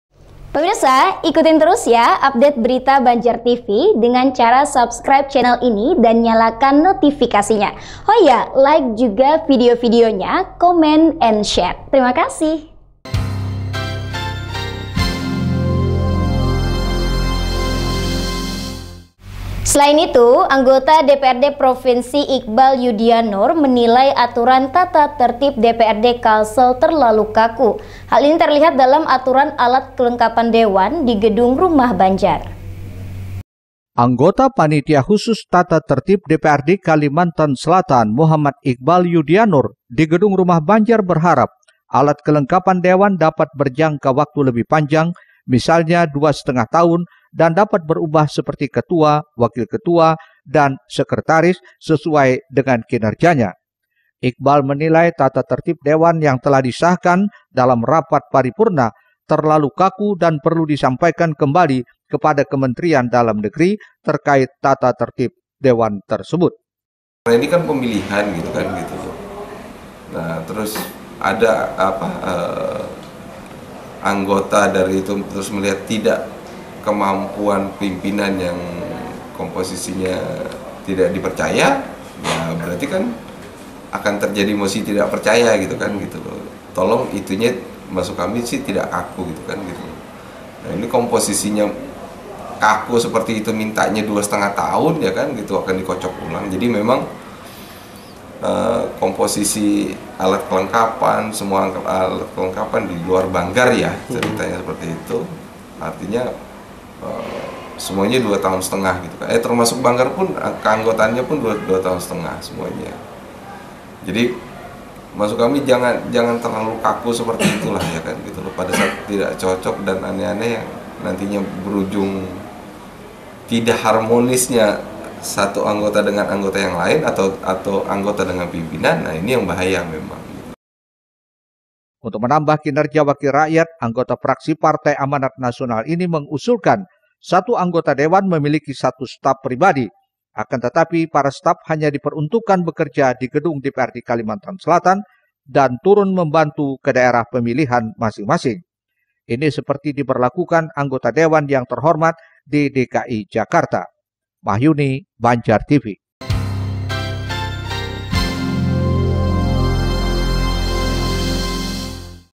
TATA TERTIB DPRD KALSEL DIANGGAP TERLALU KAKU
Anggota DPRD Provinsi, Iqbal Yudianoor menilai aturan tata tertib DPRD Kalsel terlalu kaku. Hal ini terlihat dalam aturan alat kelengkapan dewan di Gedung Rumah Banjar.